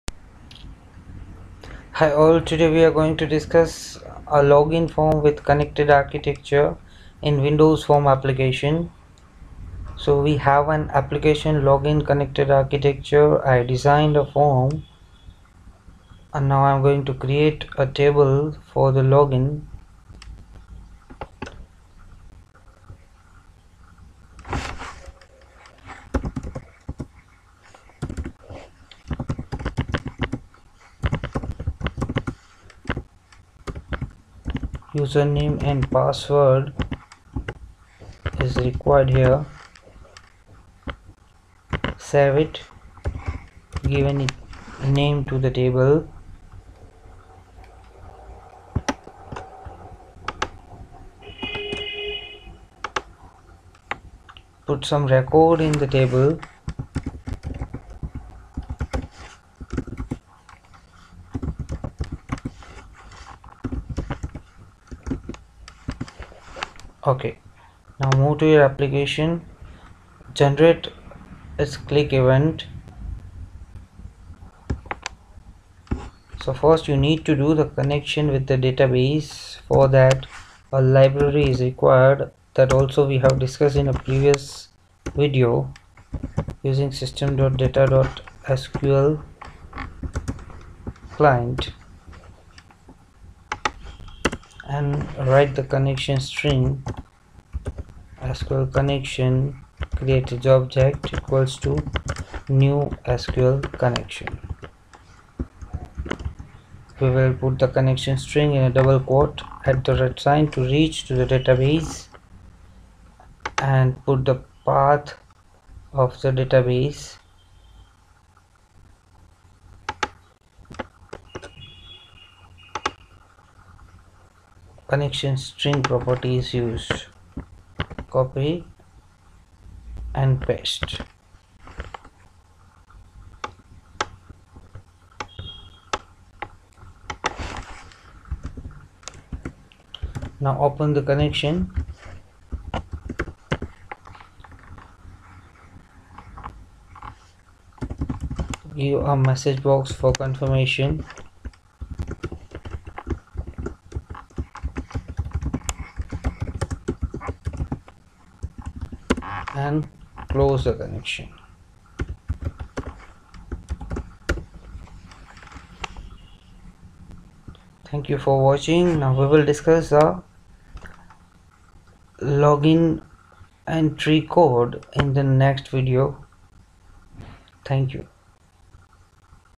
Login form with connected Architecture part-1
Login Form with connected architecture part-1 video , creation of database , table creation , connection with database is covered. also SQL Server 2014 as a database is used.